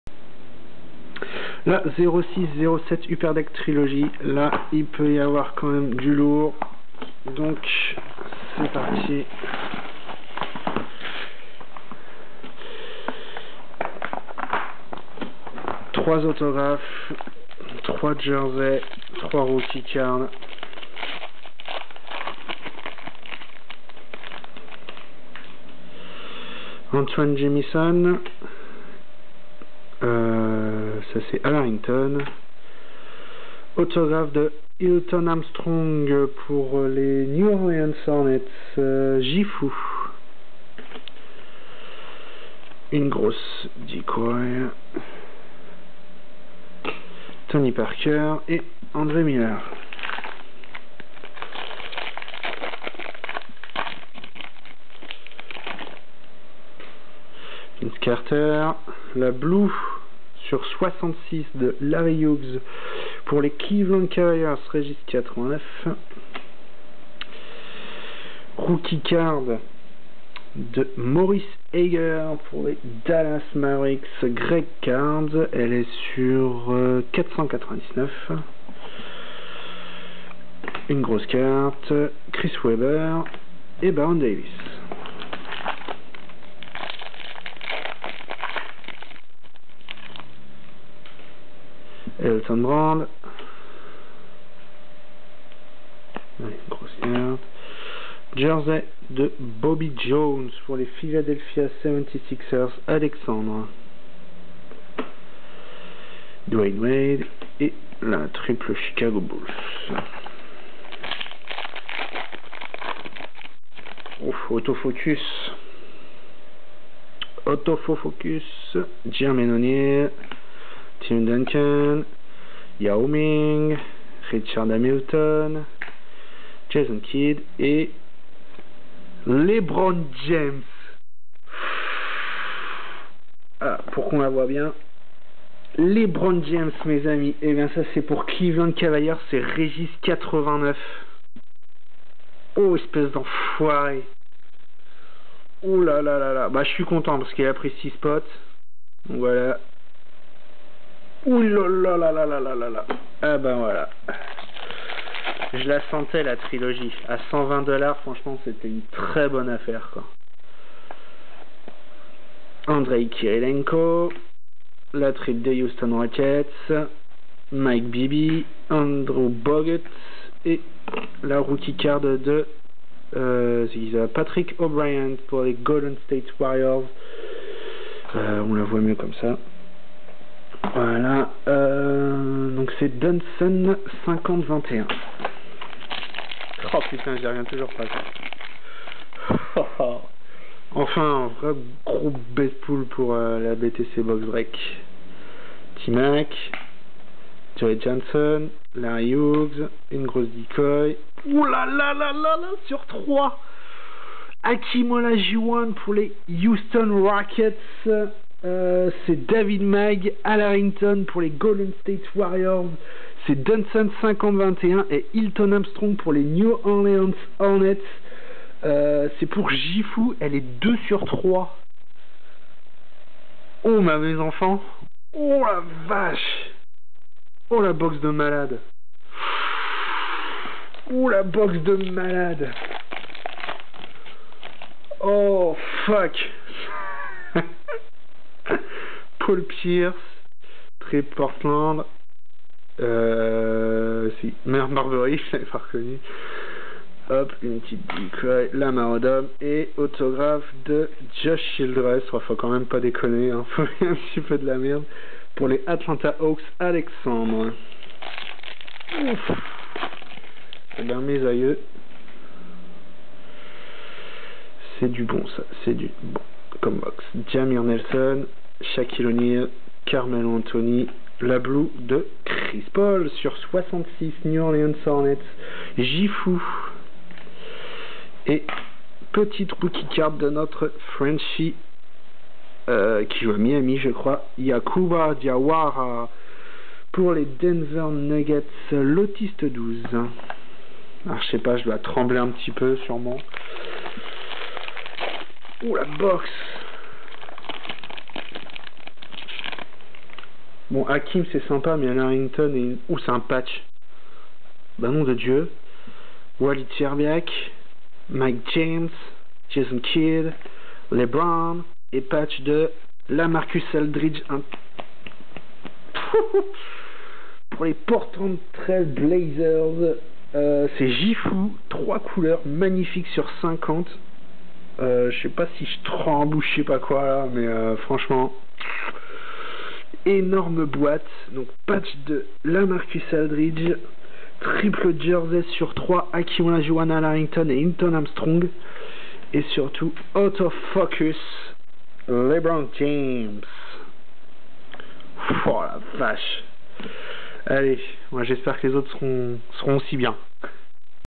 2006-07 UPPER DECK TRILOGY BTC BOXES BREAK SPECIAL EDITION MOJO LEBRON + BIG MOJO
2006-07 Upper Deck Trilogy #104 Maurice Ager RC /499 $5.00
2006-07 Upper Deck Trilogy #105 Patrick O'Bryant RC /499 $5.00
2006-07 Upper Deck Trilogy #131 Yakhouba Diawara RC /499 $5.00
2006-07 Upper Deck Trilogy Auto Focus AFLJ LeBron James SP $250.00 MOJO
2006-07 Upper Deck Trilogy Blue #11 Larry Hughes /66 $2.50
2006-07 Upper Deck Trilogy Blue #38 Chris Paul /66 $6.00
2006-07 Upper Deck Trilogy Generations Future Memorabilia FMBJ Bobby Jones $8.00
2006-07 Upper Deck Trilogy Generations Future Patches FMLA LaMarcus Aldridge /50 (3 colors) $20.00
2006-07 Upper Deck Trilogy Generations Future Signatures FSHA Hilton Armstrong $10.00
2006-07 Upper Deck Trilogy Generations Past Present and Future Memorabilia Autographs GMAOHA Olajuwon / Harrington / Armstrong /3 N/A MOJO
2006-07 Upper Deck Trilogy Generations Present Signatures PRSJC Josh Childress $10.00